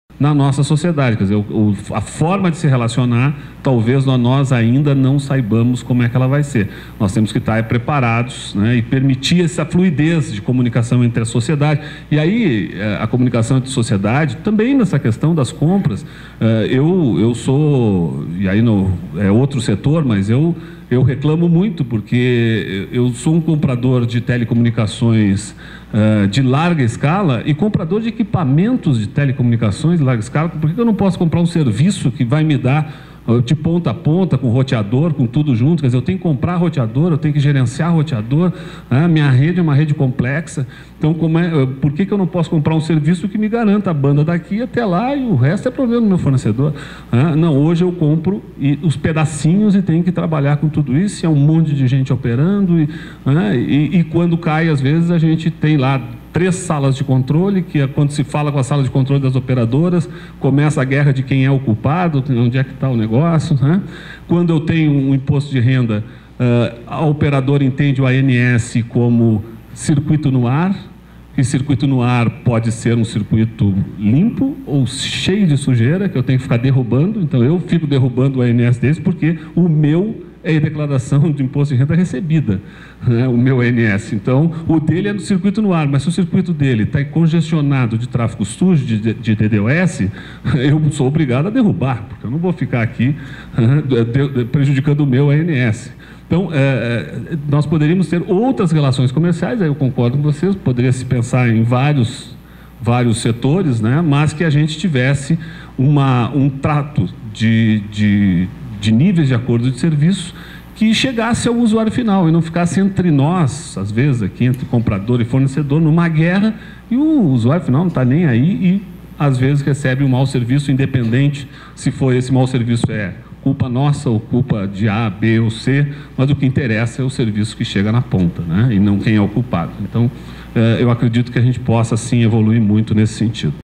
Serpro cita Teles para mostrar que modelo de compra precisa mudar
Ao participar do Forum Sulamericano de Líderes de TI de Governo, realizado pelo Grupo Ação, no dia 16 de outubro, em Brasília, o presidente do Serpro, Marcos Mazoni, disse que fornecedores e governo precisam rever seus modelos de negócios.  Mazoni citou, claramente, os contratos com as teles. "Tenho que comprar de várias, comprar equipamentos de telecom. Por que não posso comprar o serviço e cobrar a entrega dele com qualidade? Hoje temos que ter vários e há sempre a busca de 'culpados' quando há falhas", destaca o executivo.